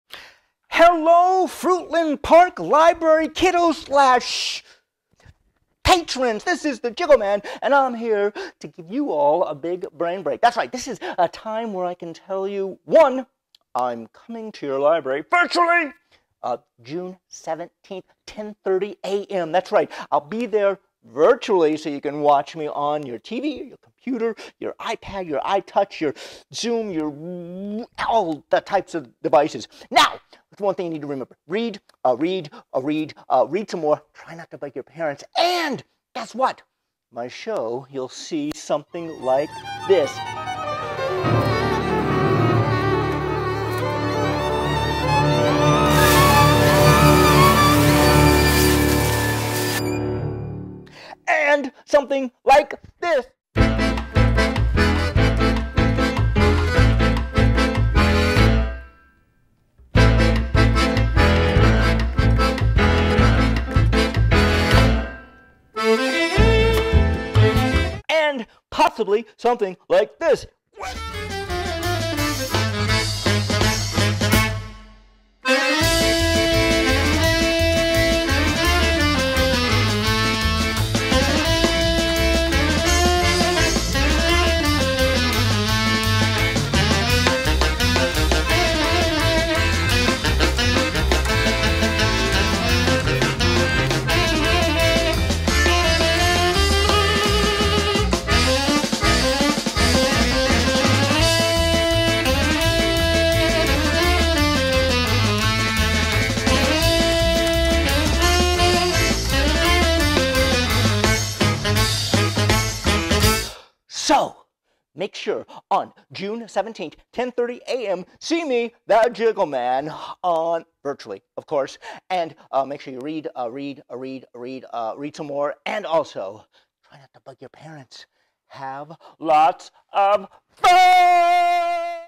Fruitland Park Library BRAIN BREAK
JiggleMan is Wild!
A purely fun show; great for all ages; audience participation; appeared seven times on The Late Show with David Letterman and performs at schools, libraries and festivals throughout the world.

Music composed by Peter Bufano and performed by Cirkestra